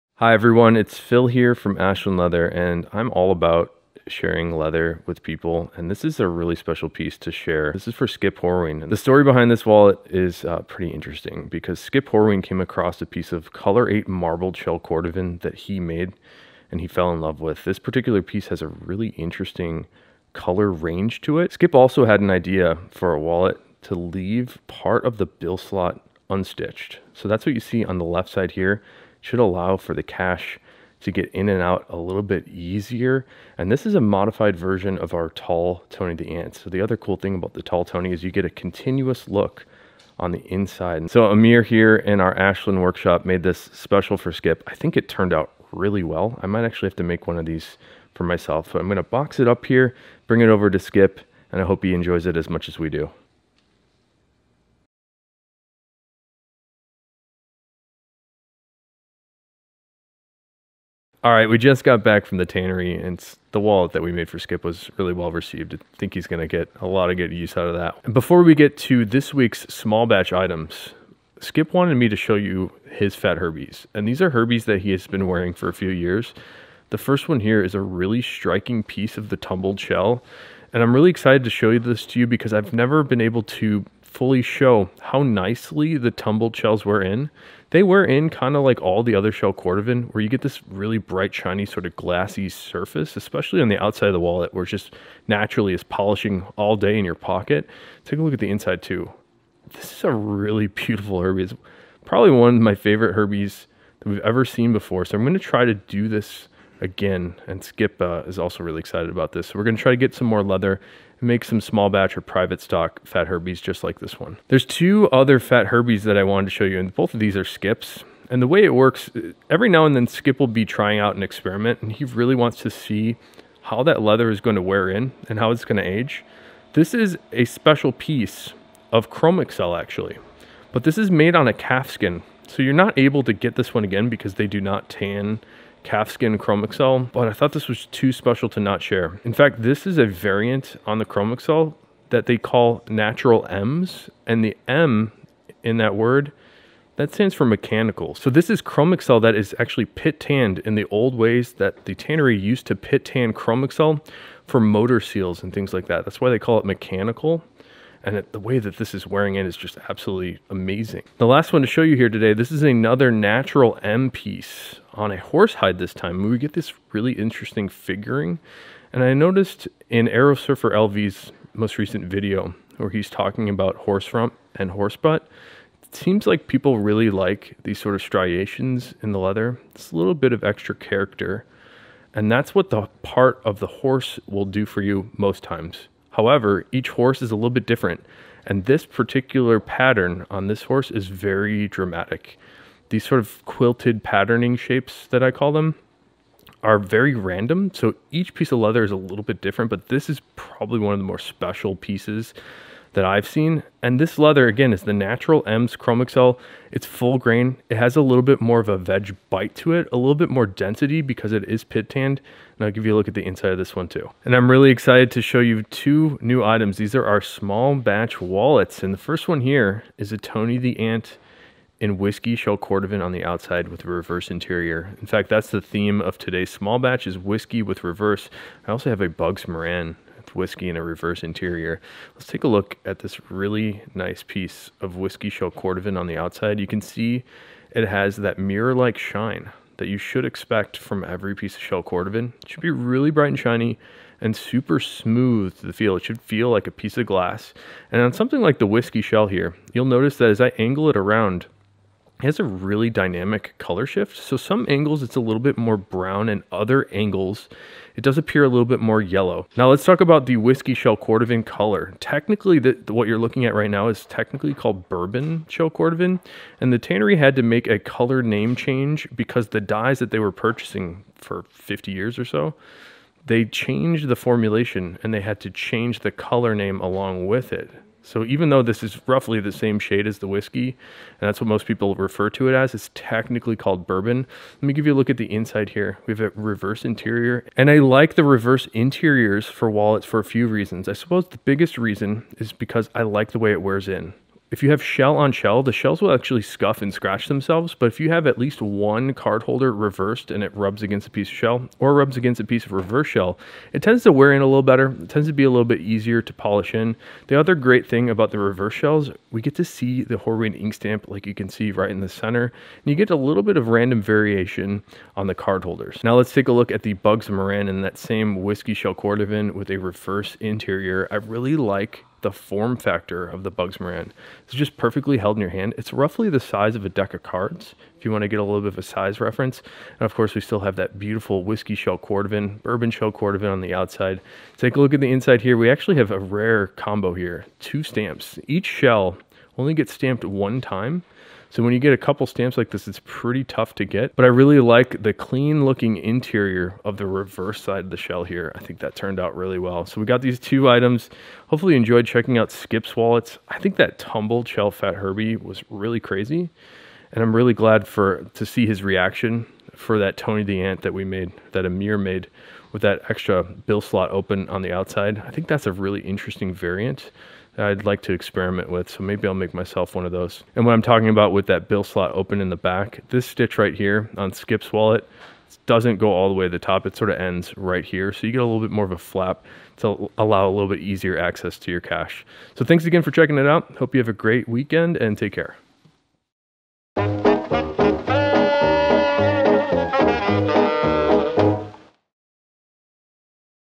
Skip Horween's Wallets + New Small Batch
Made a special wallet for Skip today. New small batch items in rare whiskey shell!